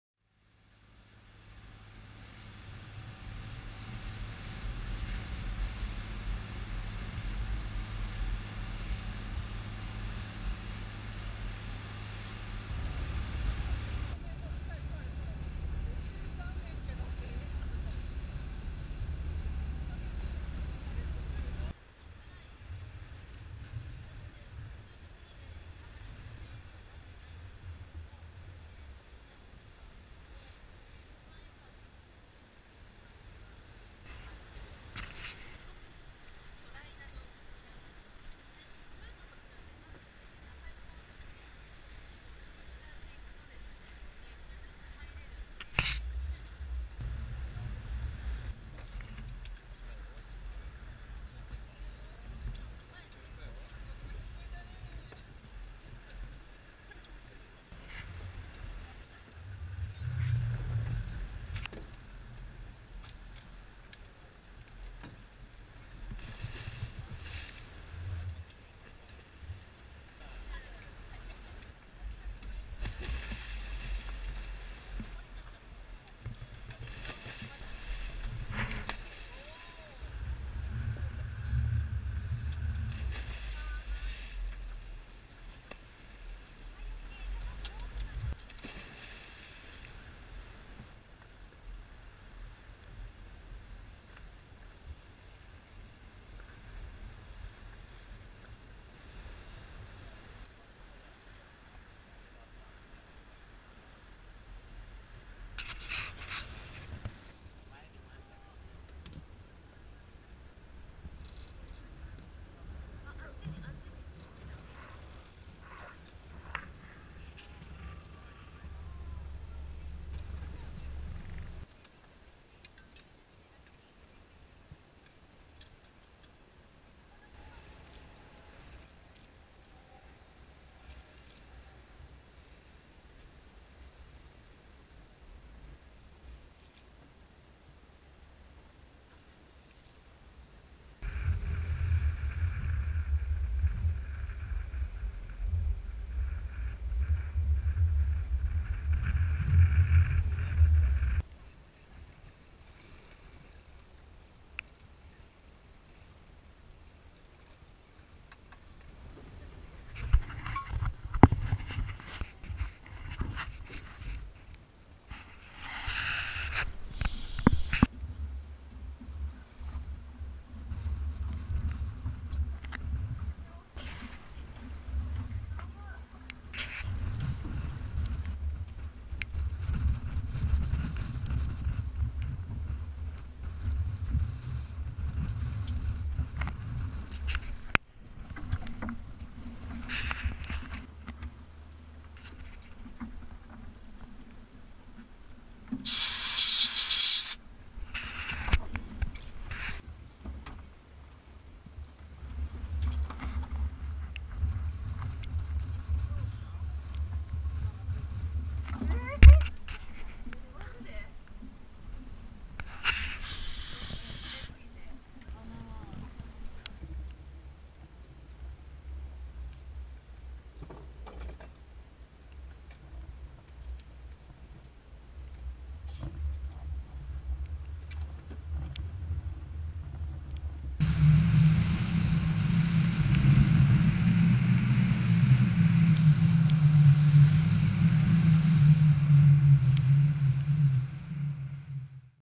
14 11 16
青空も見えたりしていますが、曇り空で帰りは小雨…
久々にマンタ登場！！２本目移動中にマンタ発見し
スノーケリング！！ついでにその場所でダイビング！！でもマンタ居らず…
ですが３本目はマンタ狙いで、水面でマンタ食事中なので、近くに止め
ダイビング＆スノーケリング！！皆さん見れたのでホッ！！としています！！明日も見られるかな～！？